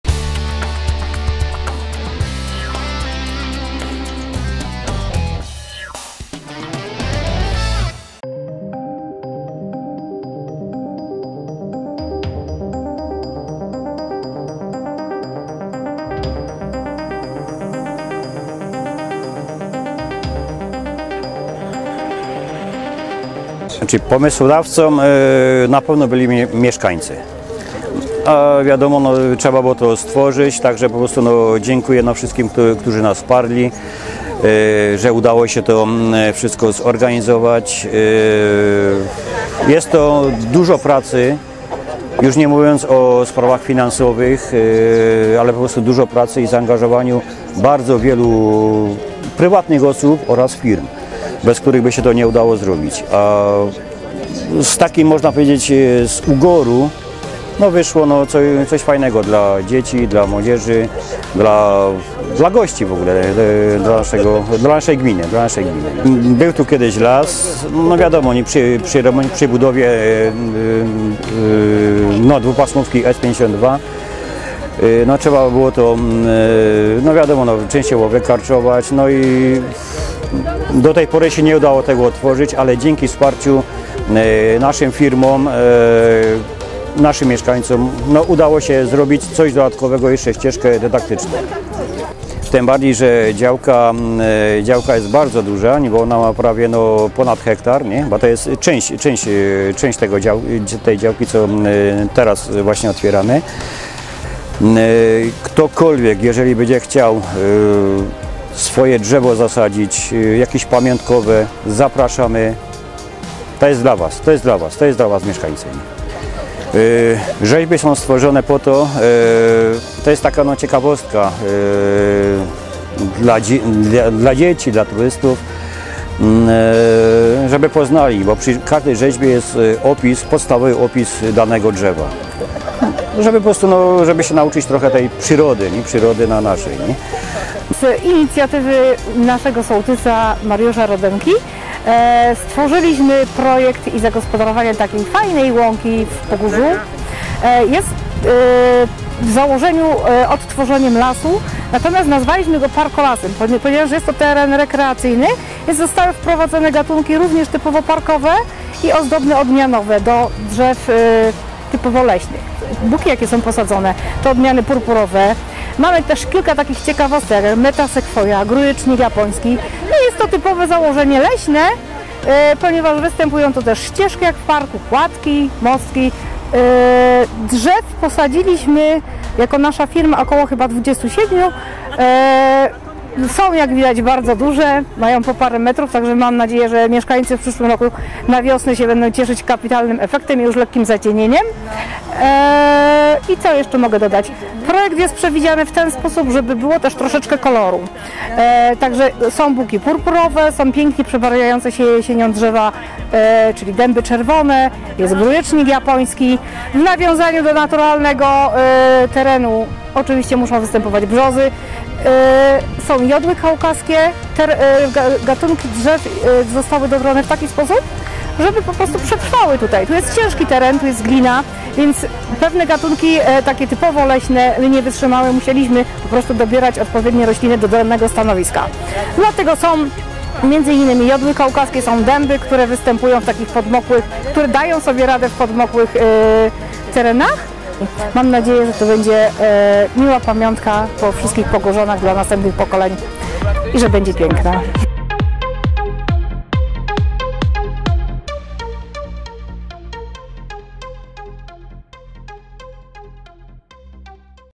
Powstaje Las w Pogórzu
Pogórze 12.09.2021
Otwarcie Ścieżki Edukacyjno-Dydaktycznej